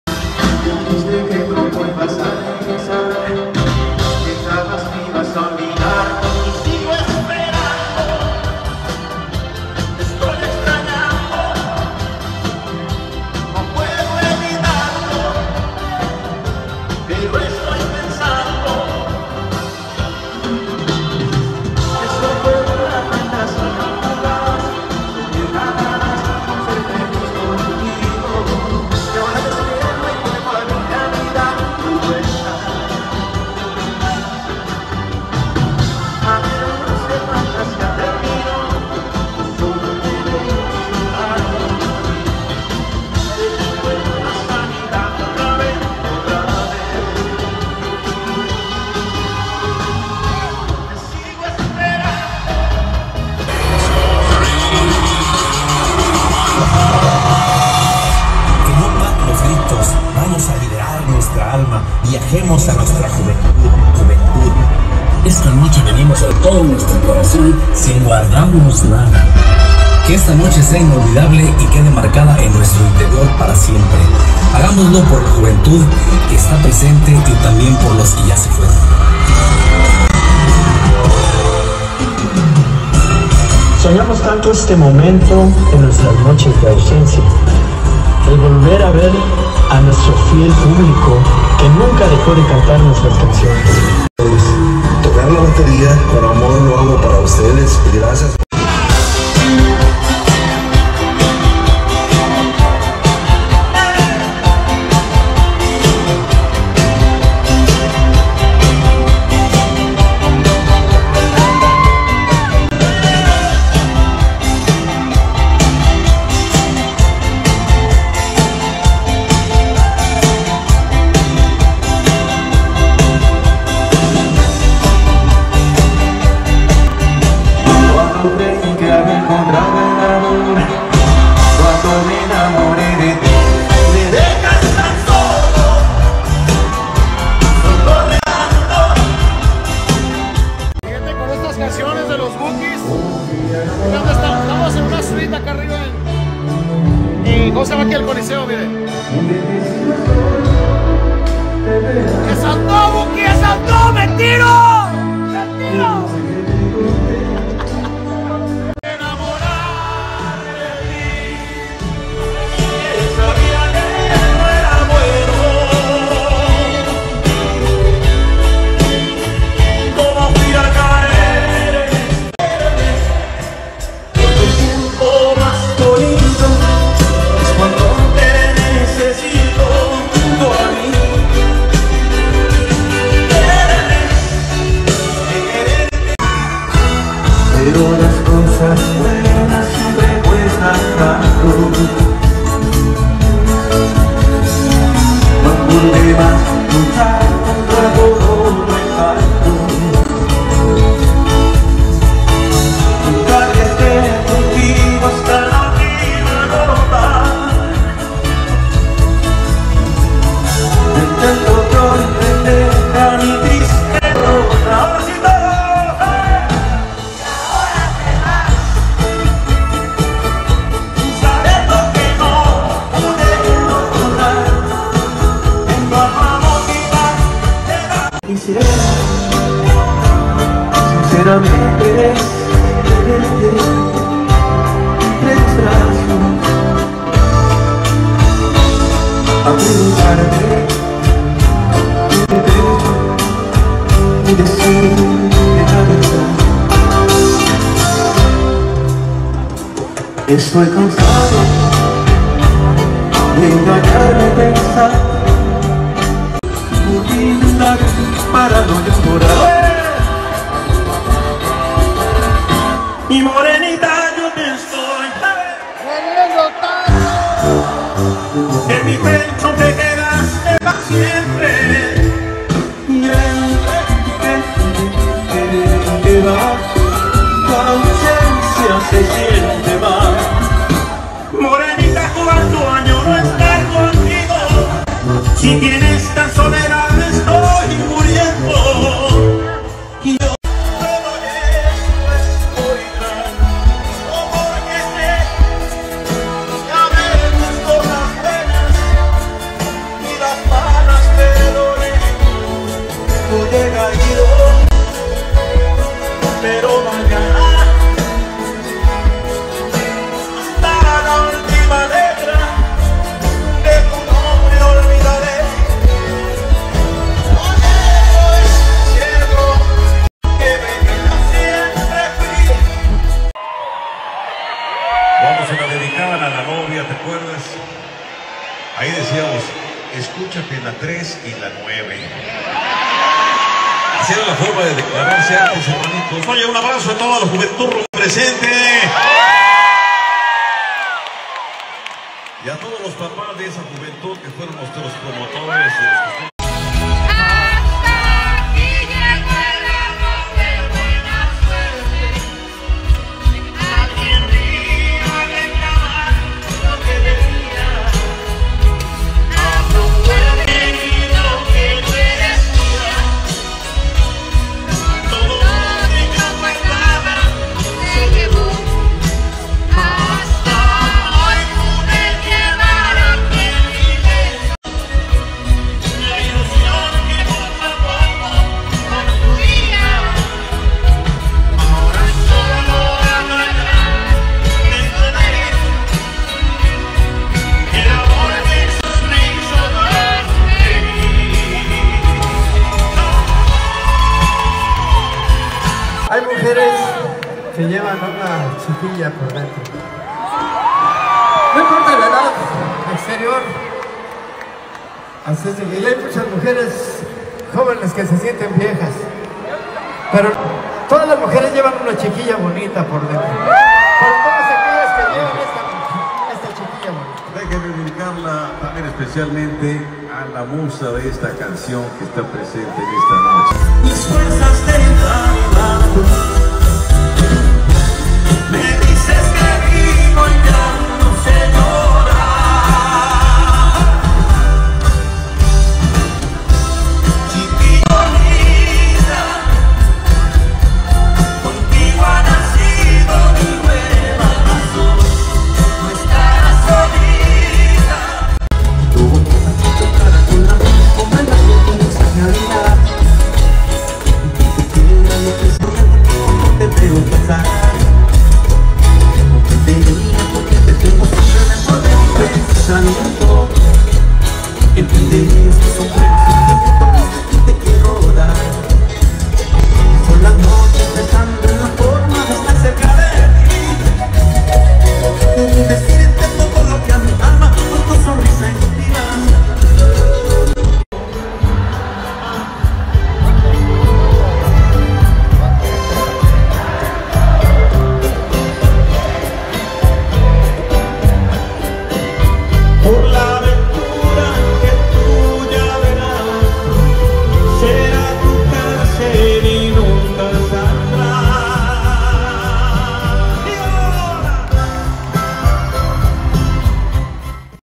Los Bukis desde Los Ángeles Memorial Coliseum, Betty Solís, Lorenzo Méndez, Sold Out 70,000 Fans
Los Bukis desde Los Ángeles Memorial Coliseum, Bety Solis, Lorenzo Mendez, Sold Out 70,000 Fans

Cristy y Marco dan noticia sobre venta de boletos para Los Bukis en Michoacán y Mas..

Muchos saludos y Bendiciones donde quiera que estés, Gracias por visitar mi canal😍💞